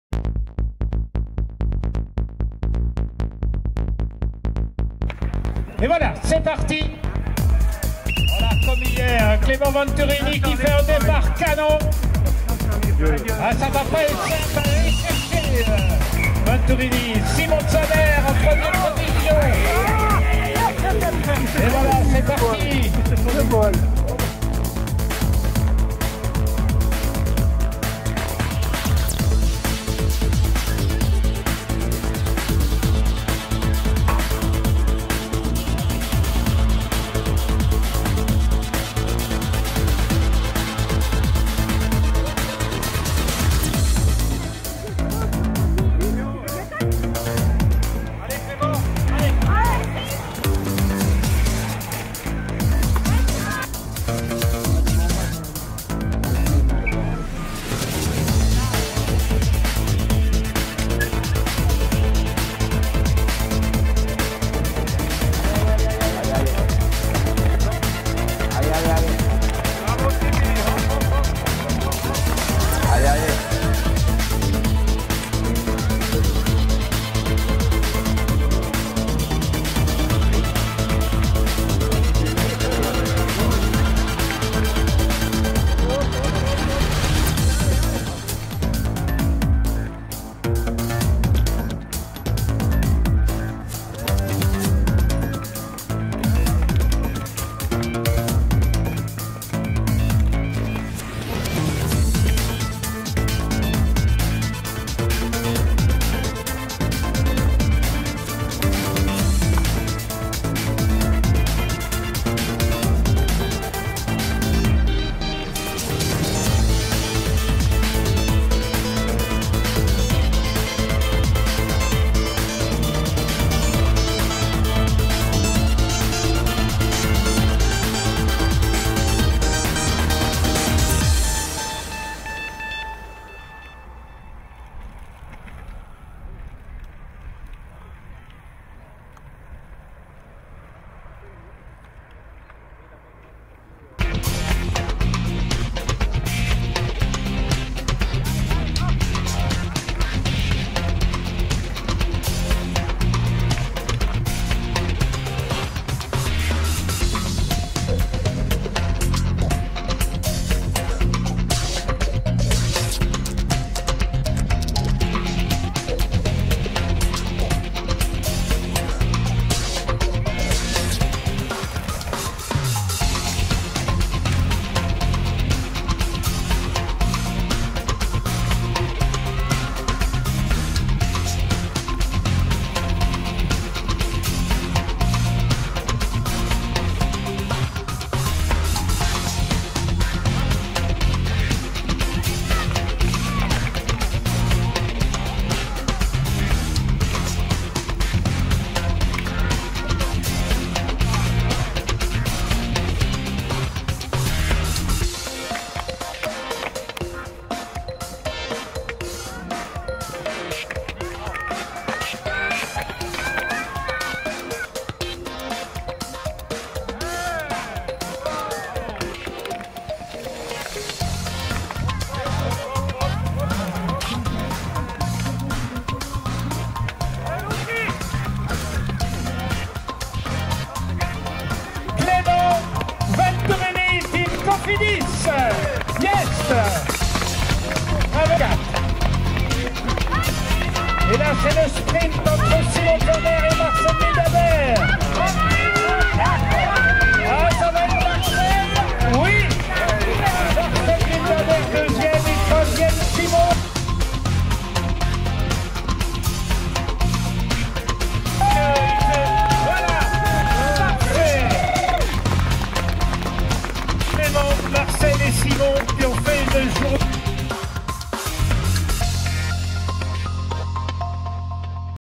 2017 Cyclocross Herren Sion
Cyclocross Herren Sion 2017
Created with MAGIX Video Pro X7
1. VENTURINI Clément (FAR)
2. WILDHABER Marcel (SUI)
3. ZAHNER Simon (SUI)